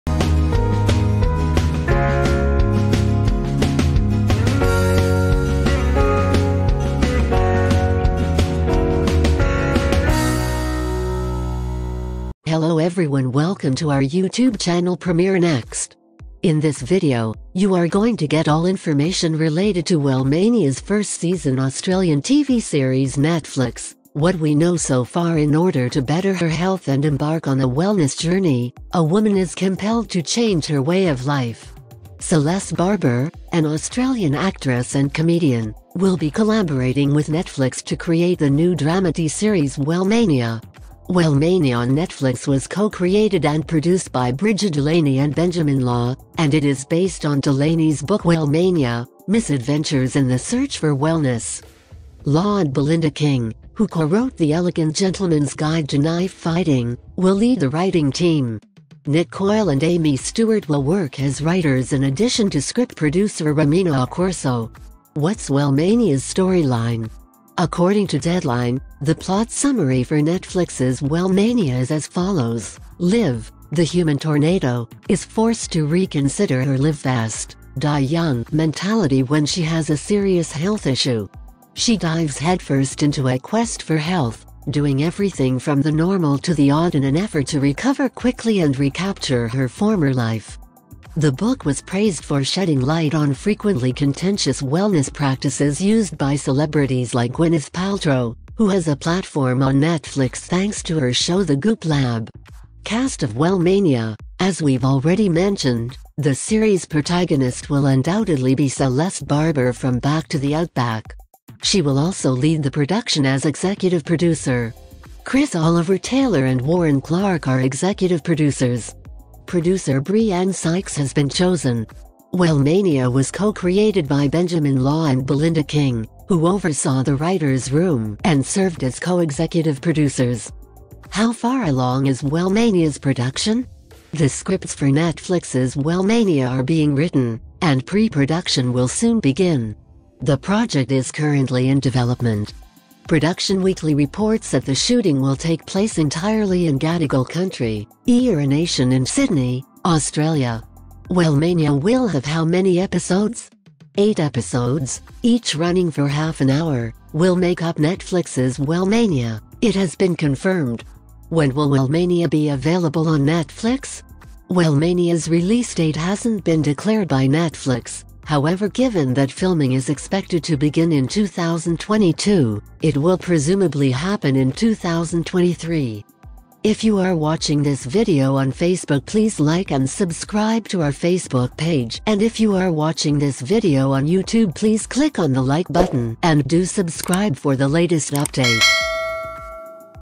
Wellmania Season 1: Netflix Release Date & Cast Updates - Premiere Next
Wellmania Season 1: Netflix Release Date & Cast Updates
in this video, you will get the info related to Wellmania Season 1 Release Date, Cast, and Key Details.

Technology updates
Netflix Series
Netflix Movies
Premiere Next